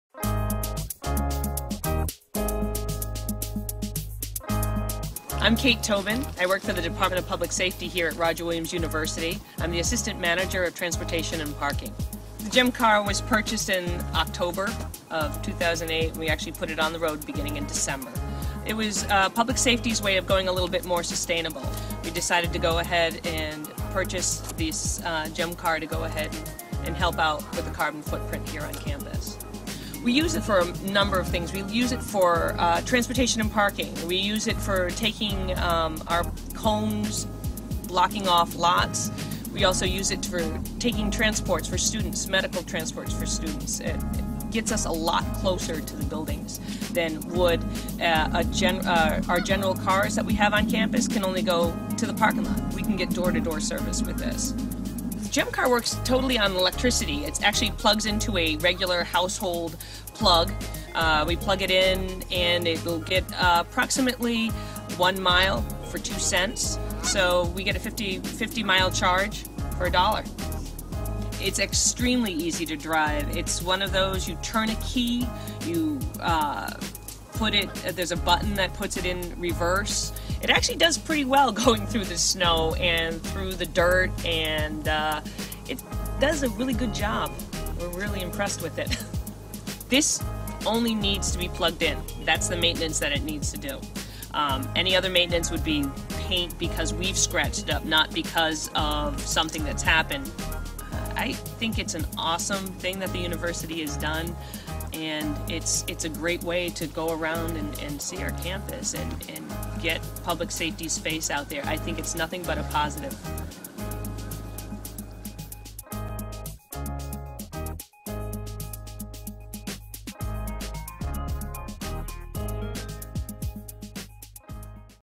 RWU GEM Car
This video was shot during the 2010 Roger Williams University Planet Forward cycle, using a Canon HF-S10 camera on campus in Bristol, Rhode Island.  This video takes you on a ride-a-long to have a glimpse at the electric vehicle that is used to patrol campus and enforce parking protocols.  The video was edited using Final Cut Pro, and the music track was created using Garage Band.